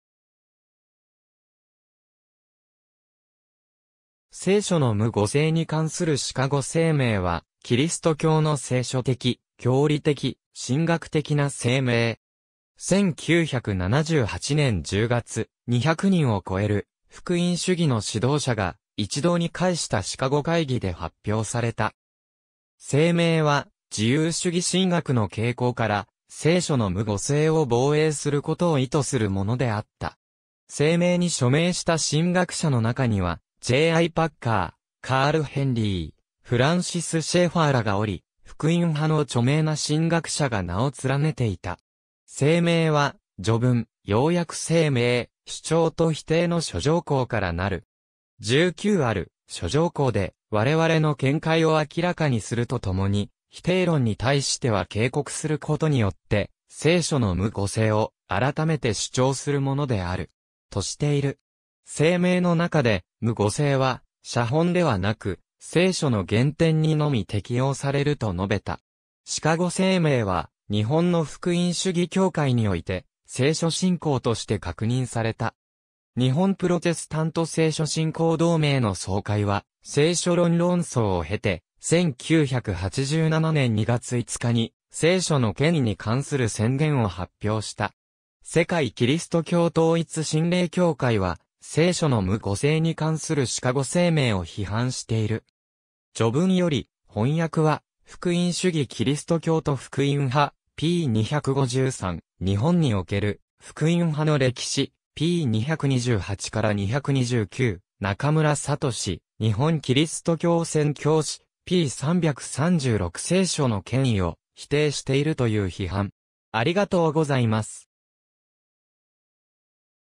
聖書の無誤性に関するシカゴ声明
キリスト教信仰宣言、信経、信条、信仰告白
聖書の無誤性に関するシカゴ声明（せいしょのむごせいにかんするしかごせいめい、The Chicago Statement on Biblical Inerrancy）は、 キリスト教の聖書的、教理的、神学的な声明。
1978年10月、 200人を超える福音主義の指導者が一堂に会したシカゴ会議で発表された。
声明は、自由主義神学（リベラル派）の傾向から、聖書の無誤性（Biblical inerrancy ）を防衛することを意図するものであった。
声明に署名した神学者の中には、J・I・パッカー、カール・ヘンリー、フランシス・シェーファーらがおり、福音派の著名な神学者が名を連ねていた。
声明は序文、要約声明、主張と否定の諸条項からなる。
19ある諸条項で「われわれの見解を明らかにするとともに、否定論に対しては警告することによって、聖書の無誤性をあらためて主張するものである。」としている。
声明の中で無誤性は、写本ではなく、聖書の原典にのみ適用されると述べた。
シカゴ声明は、日本の福音主義教会において聖書信仰として確認された。
日本プロテスタント聖書信仰同盟（JPC）の総会は、聖書論論争を経て、1987年2月5日に聖書の権威に関する宣言を発表した。
世界基督教統一神霊協会は、聖書の無誤性に関するシカゴ声明を批判している。
^ 序文より、翻訳は『福音主義キリスト教と福音派』p.253 ^ 『日本における福音派の歴史』p.228-229 ^ 中村敏『日本キリスト教宣教史』p.336 ^ 聖書の権威を否定しているという批判